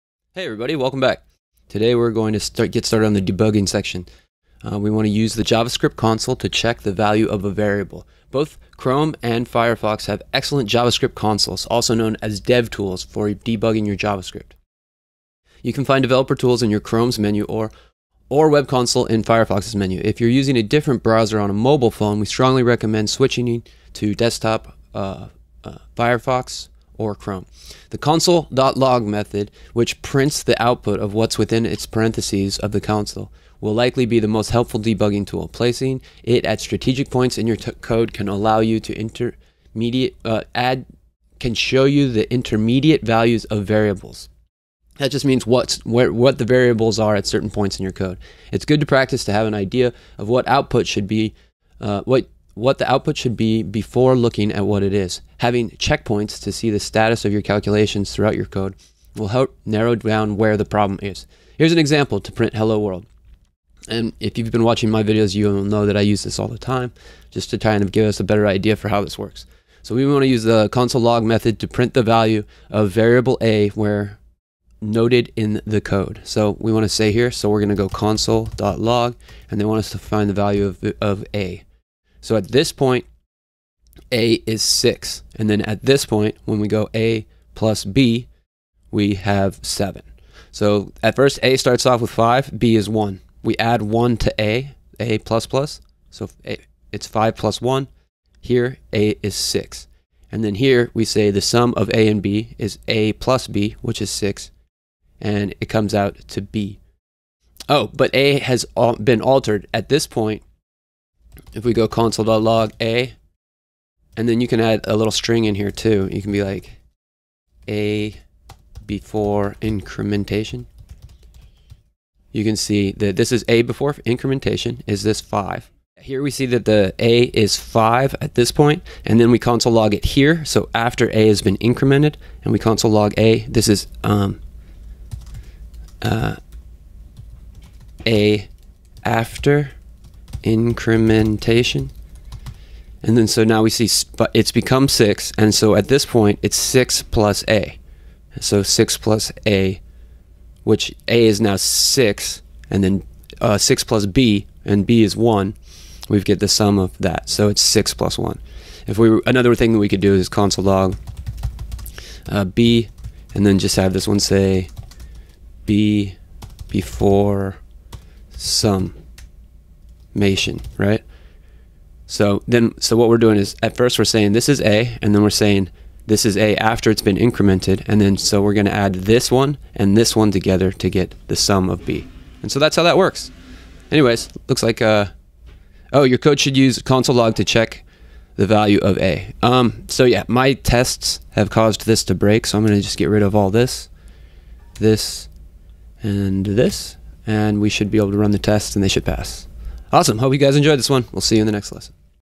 Use the JavaScript Console to Check the Value of a Variable Debugging - Free Code Camp
In this debugging tutorial we use the JavaScript console to check the value of a variable while debugging. We're exploring tools and common errors so we can identify bugs in JavaScript. This makes up part of the Free Code Camp curriculum. Enjoy!